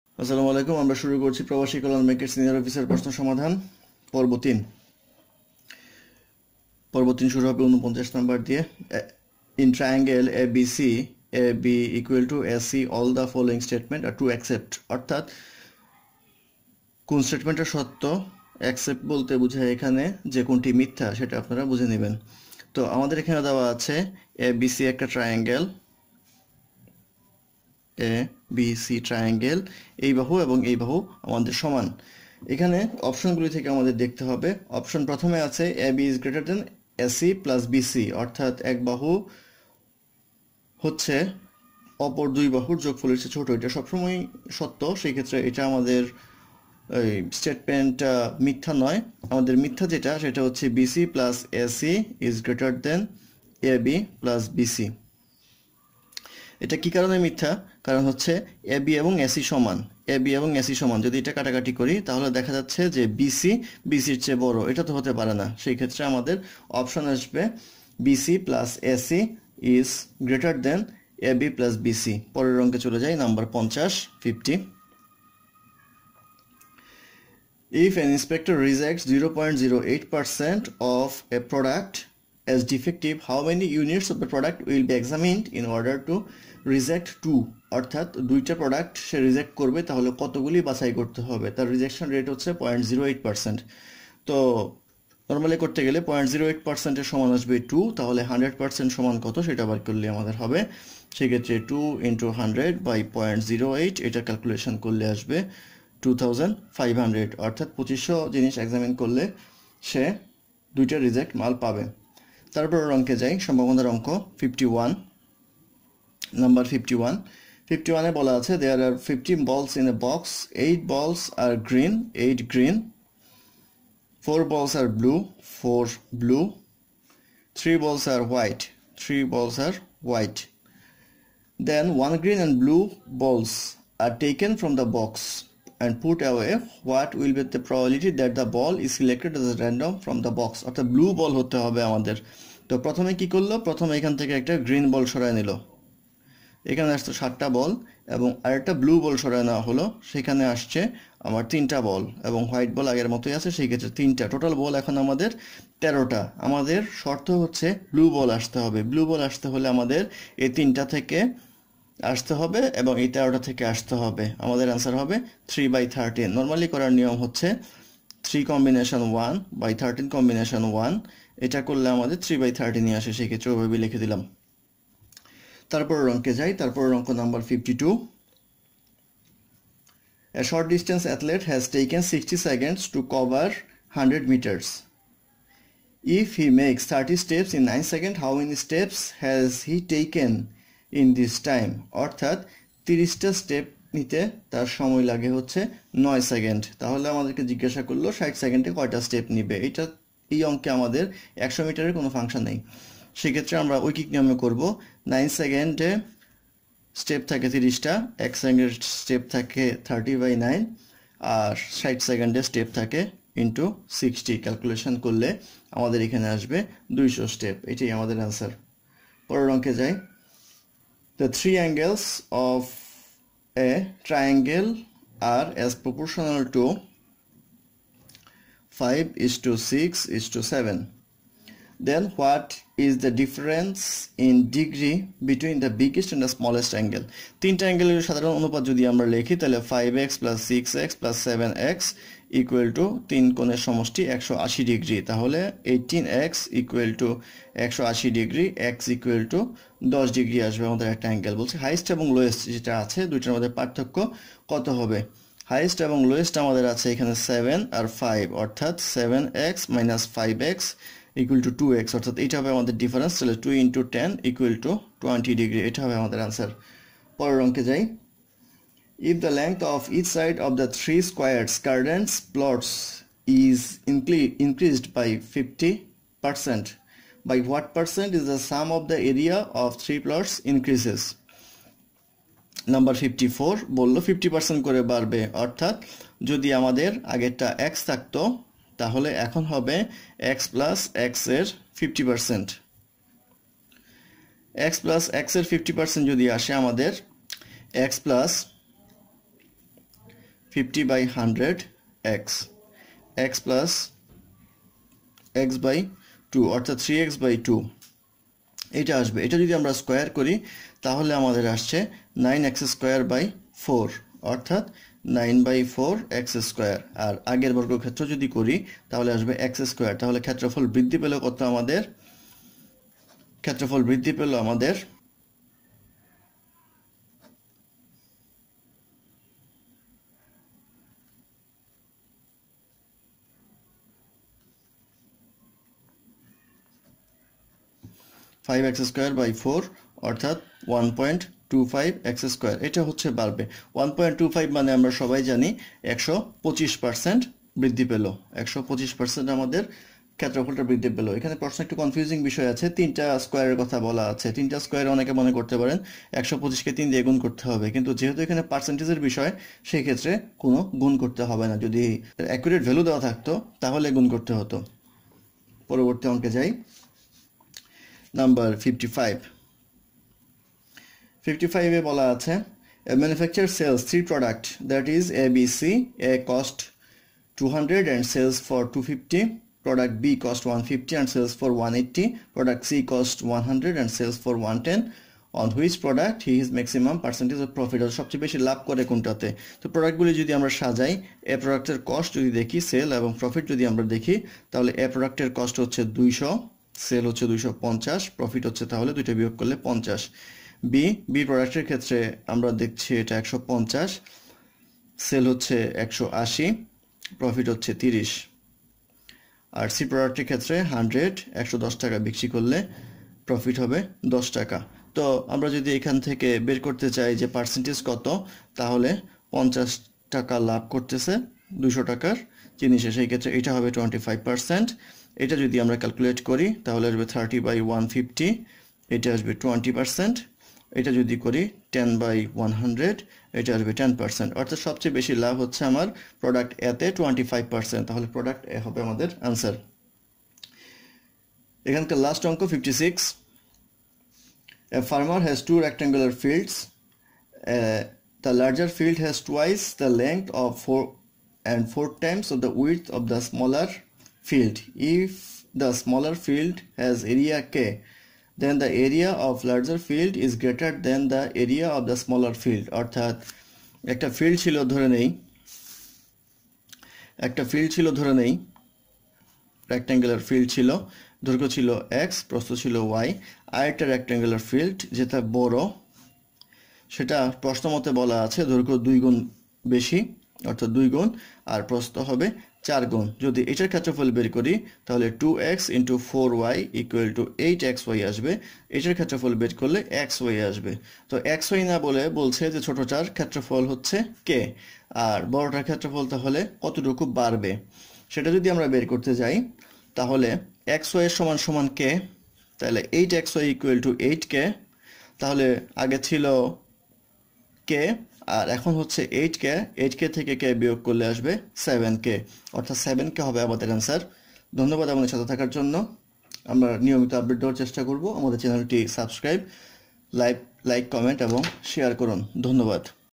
Probashi Kallyan Bank Senior Officer Math Question Solution Part 3, Exam Date 19-01-2018
mathhelpline mathhelpline Probashi Kallyan Bank Senior Officer Math Question Solution Part-3
প্রবাসী কল্যাণ ব্যাংক সিনিয়র অফিসার গণিত প্রশ্ন সমাধান পরীক্ষার তারিখ ১৯-০১-২০১৮